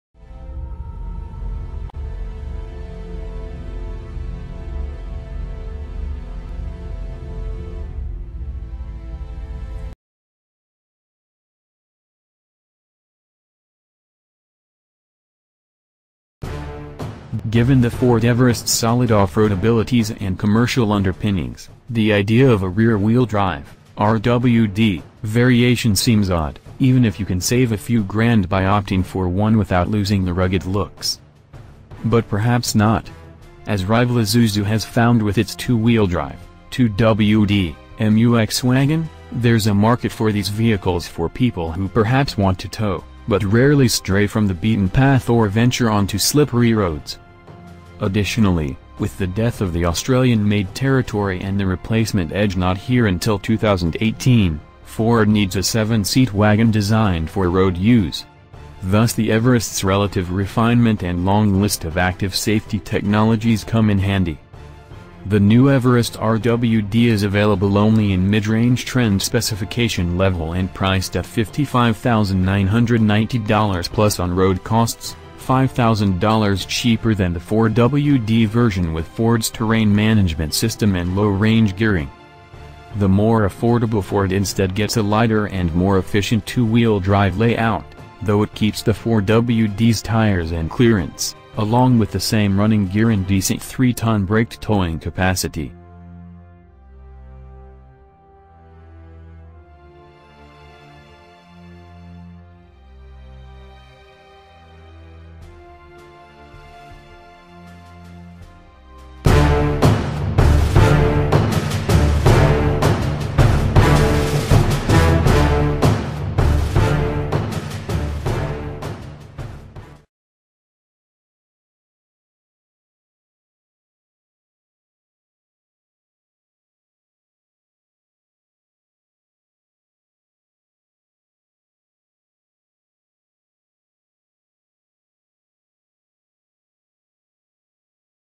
Watch Now!! 2017 Ford Everest Trend RWD Price & Interior
Watch Now!! 2017 Ford Everest Trend RWD Price & Interior -Given the Ford Everest Trend RWD solid off-road abilities and commercial underpinnings, the idea of a rear-wheel drive (RWD) variation seems odd, even if you can save a few grand by opting for one without losing the rugged looks.

 2017 Ford Everest Trend RWD $5000

Popular Post !